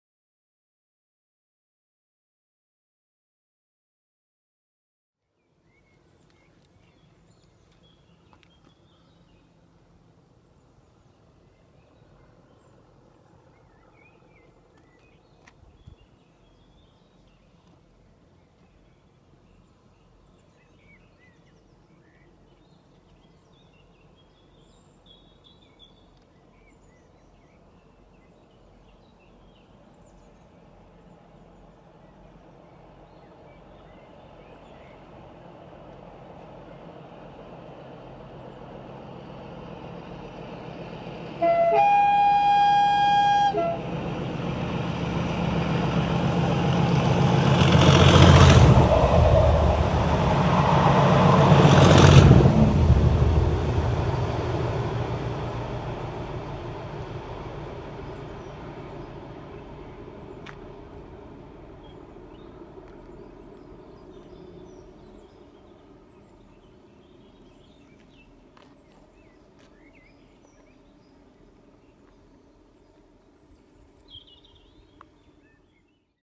Colas Rail 37116 and 37175 thunder under Bargate Lane on 1Q97
Returning from Harwich Town, 37116 and 37175 are seen thundering under Bargate Lane on 1Q97 from Ferme Park to Cambridge.